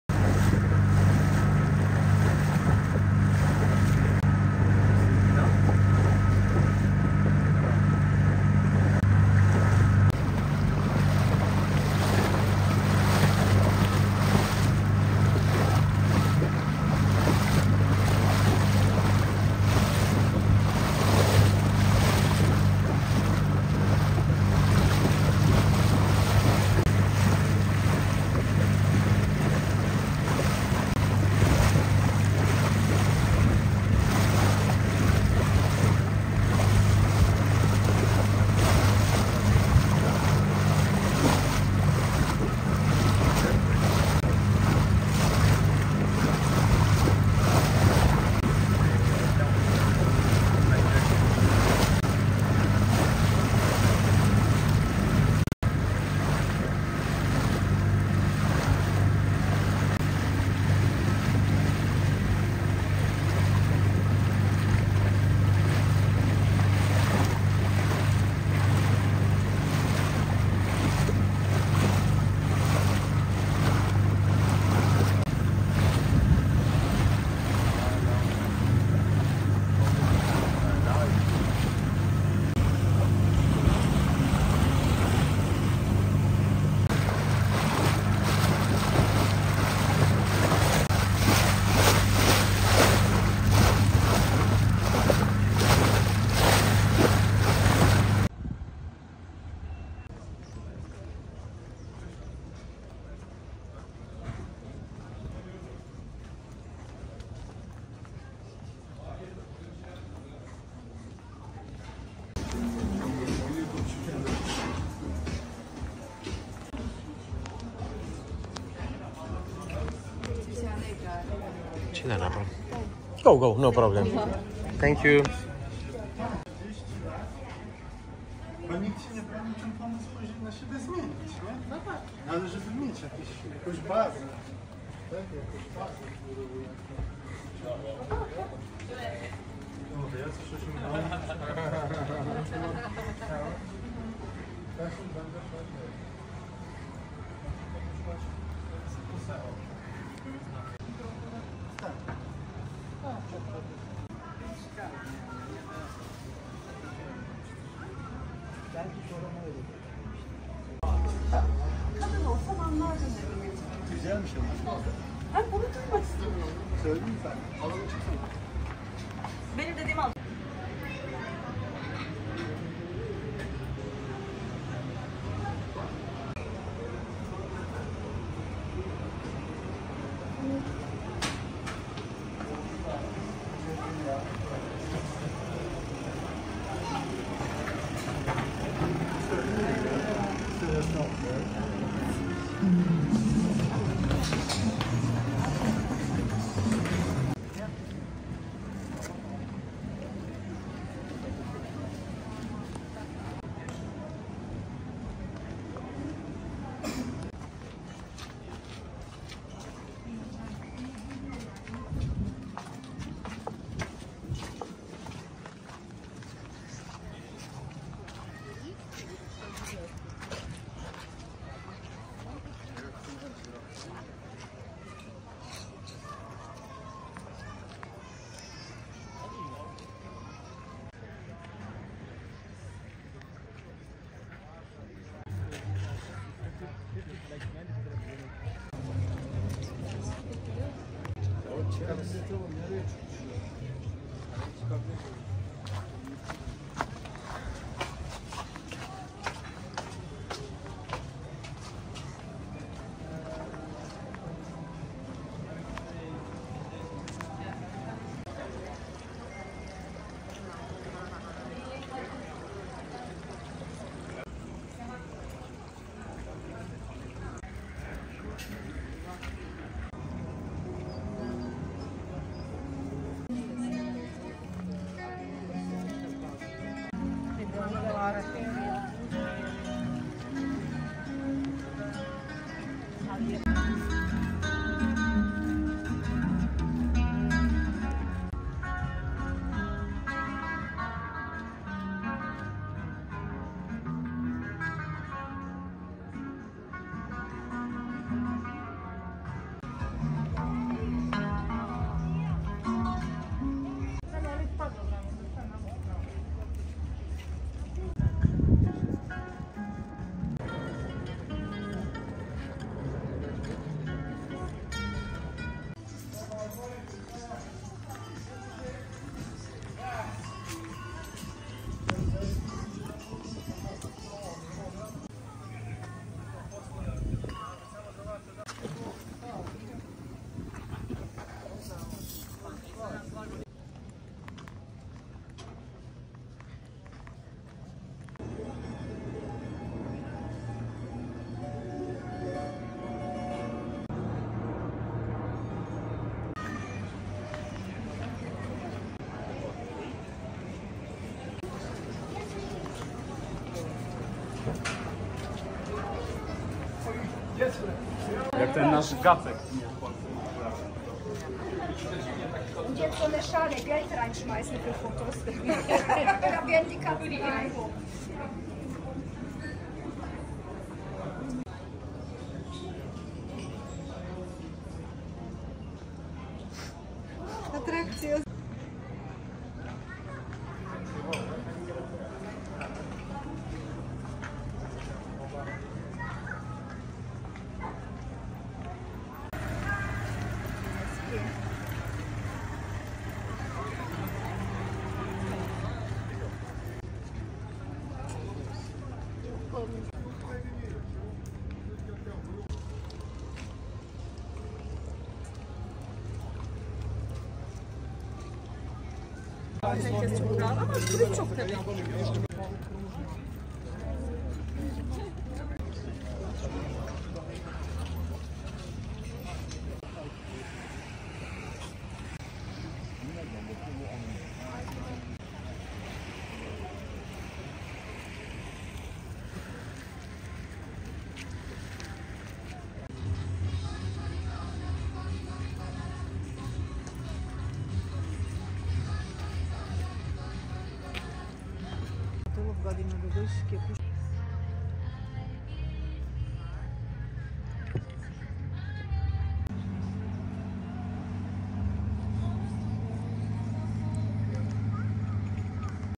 BUDVA ( old town )
Driving with boat around the old town, and walking in old town Budva - Montenegro